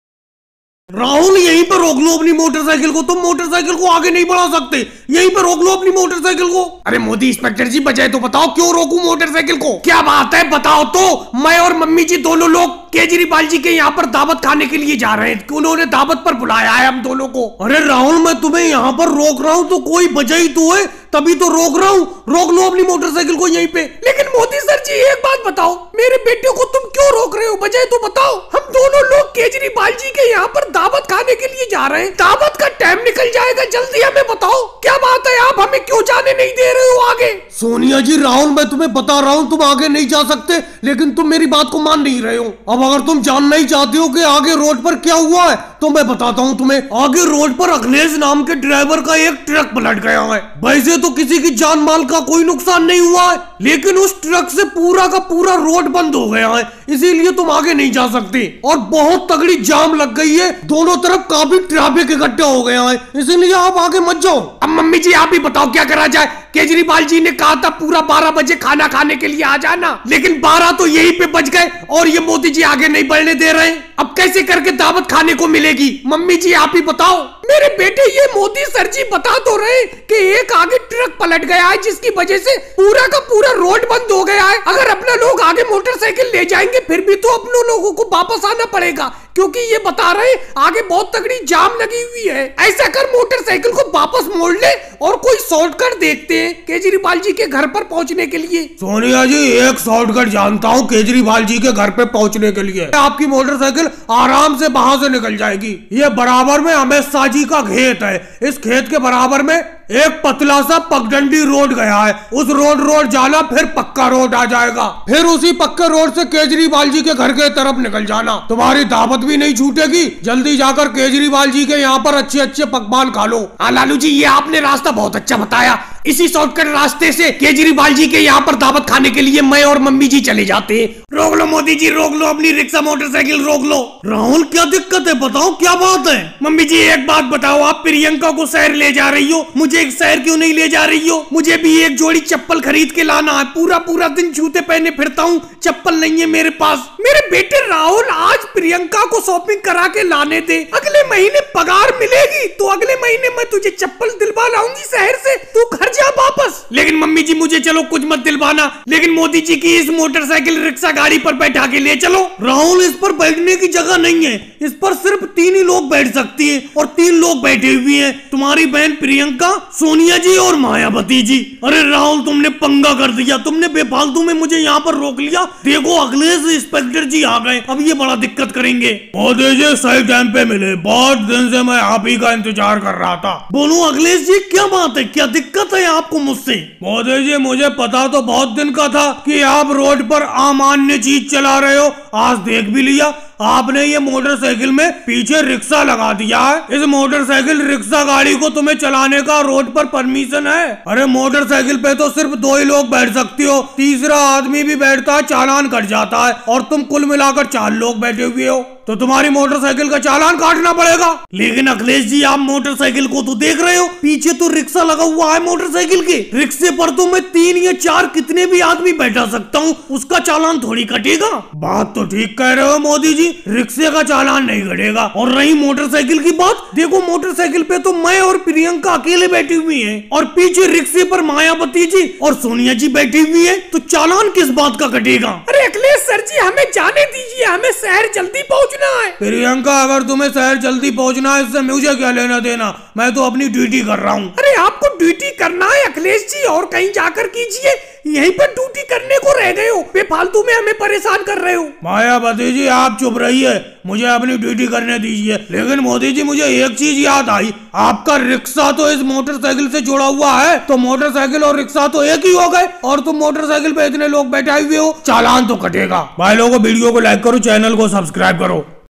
मोदी जी ने सोनिया जी और राहुल जी को मोटरसाइकिल पर जाने से रोका | Police Comedy
Narendra Modi Sonia Gandhi Lalu Prasad Yadav Rahul Gandhi Funny Comedy Video

Motorcycle comedy

Police comedy video